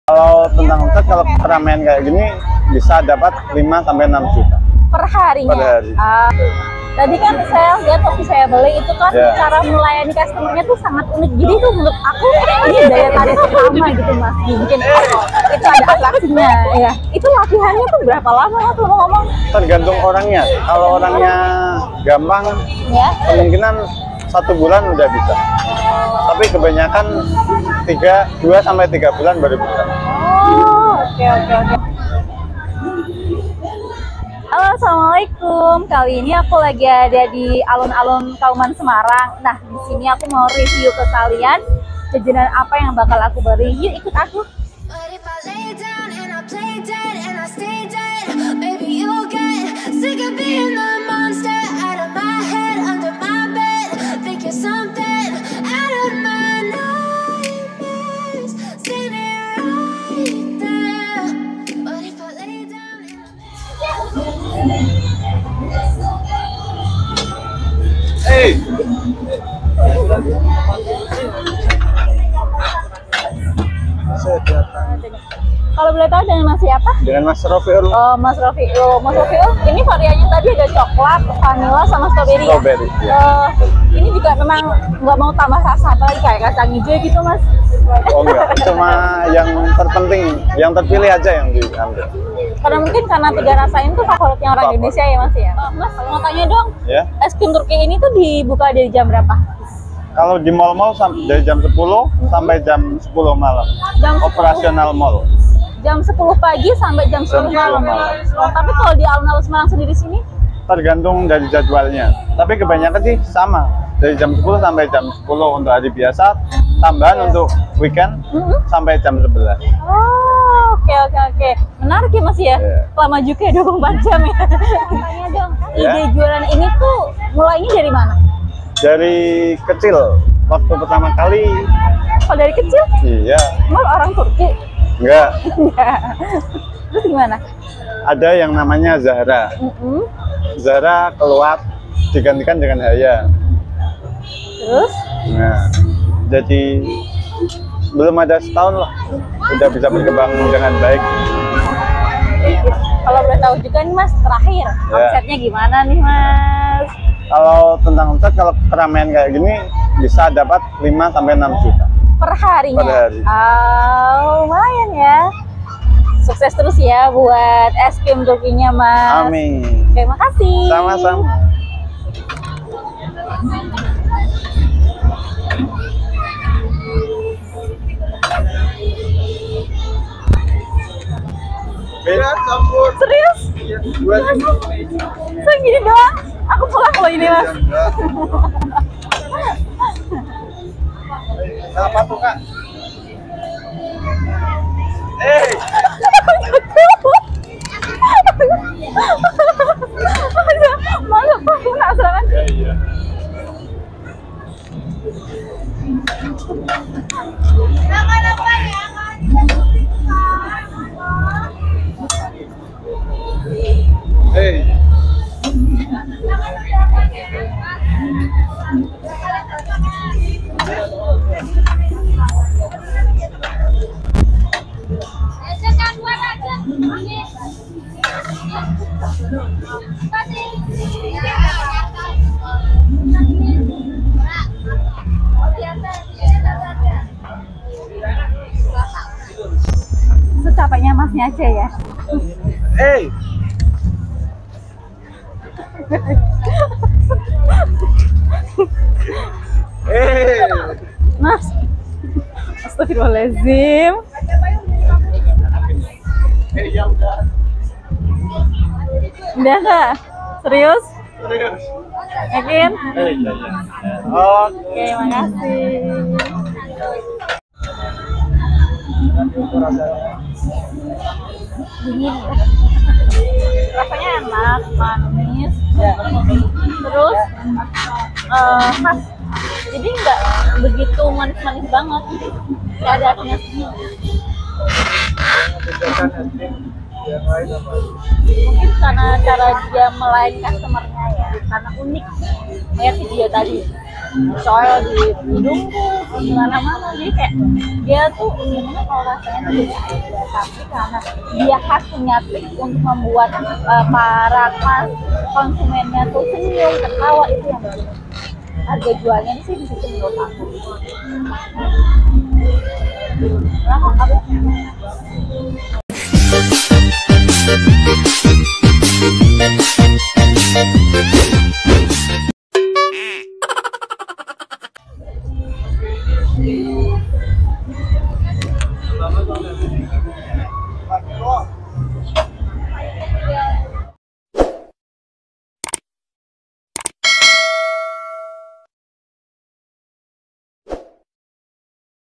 CUMAN JUALAN ES KRIM‼️SEHARI OMSET 6 JUTA, 😱 ES CREAM HAYA TURKISH DISERBU BANYAK PEMBELI‼️
ES KRIM TURKI HAYA salah satu bisnis kuliner yang menjual es CREAM, dalam melayani customer dengan cara melakukan atraksi yang bisa menghibur pelanggan. es CREAM rasanya dingin segar manis banyak disukai anak - anak.
ES CREAM TURKI HAYA
alamat: alun - alun Kauman Semarang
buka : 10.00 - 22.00
KULINER SEMARANG